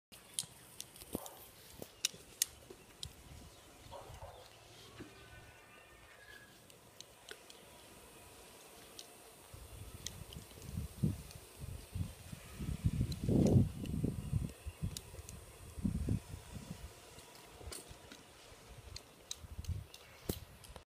small puppy playing football on his own
small puppy playing football in the garden